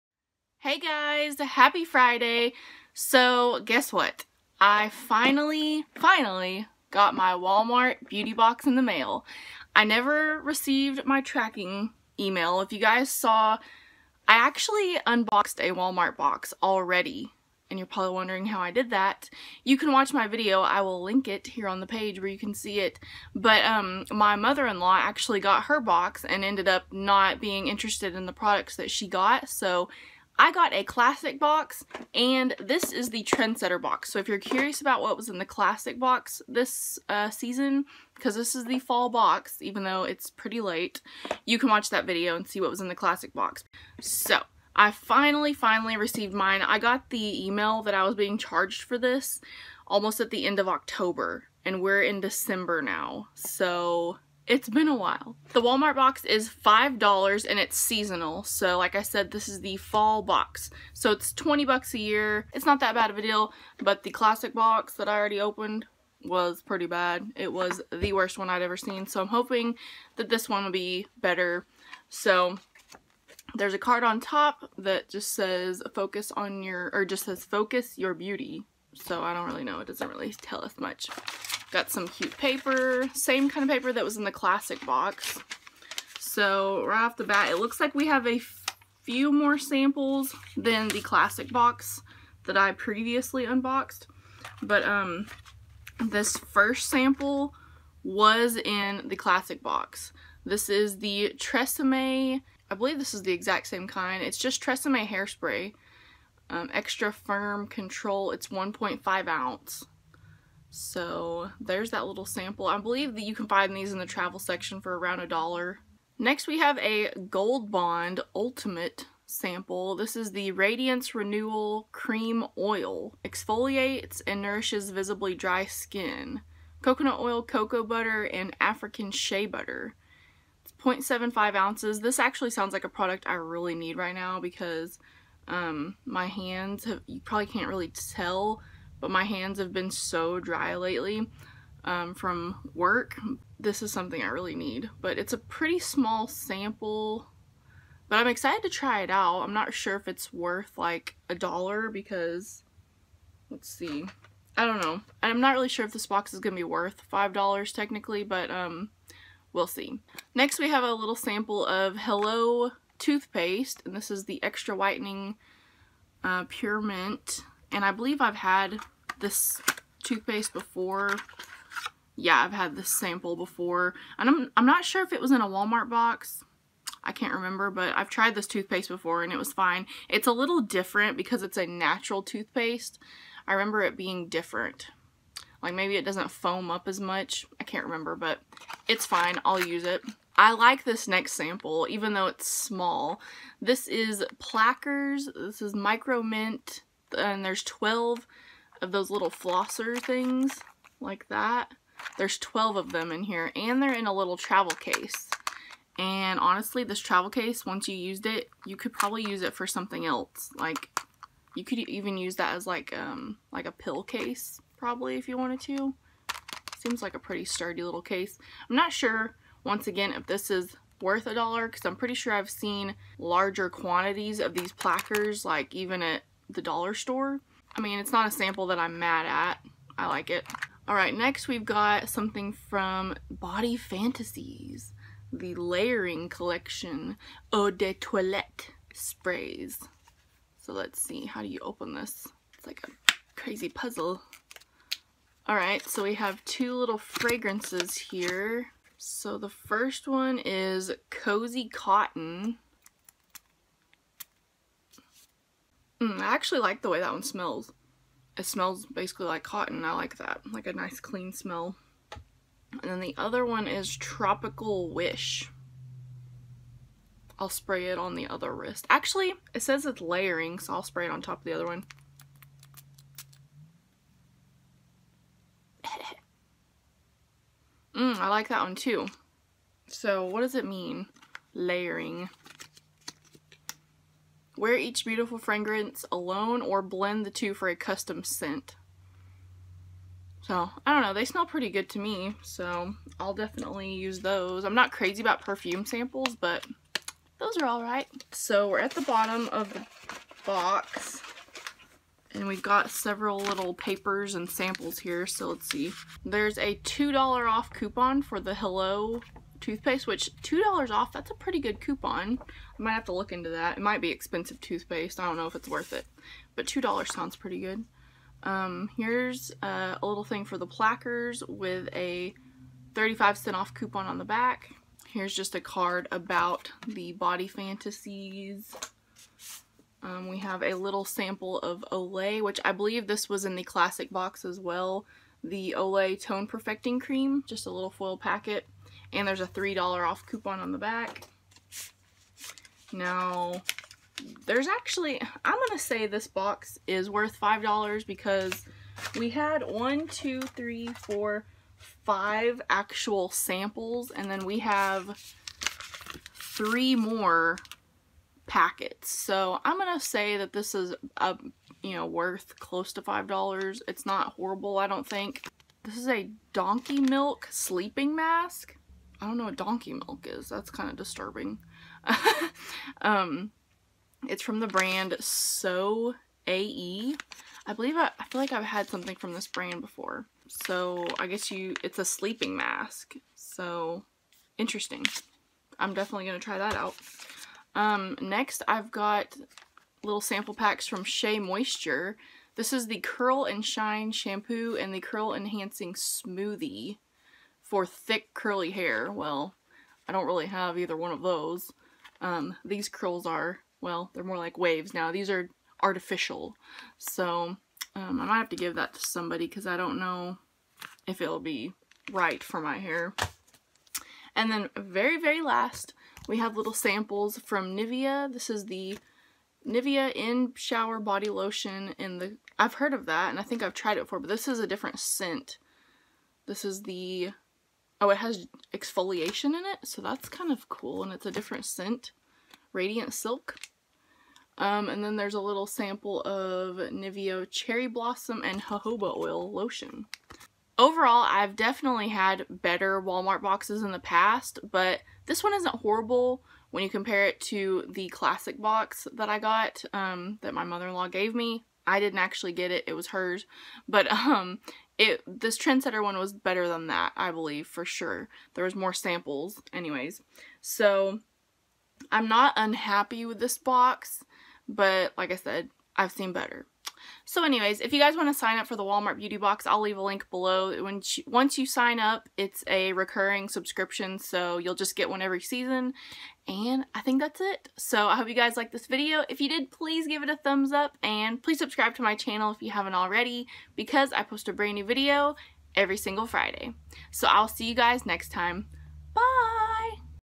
WALMART BEAUTY BOX | FALL 2017 TRENDSETTER
Sign up for the Walmart Beauty Box:

This box is seasonal and only $5! They say that the $5 you pay is just the shipping cost (the box is considered FREE). This is the Trendsetter Box.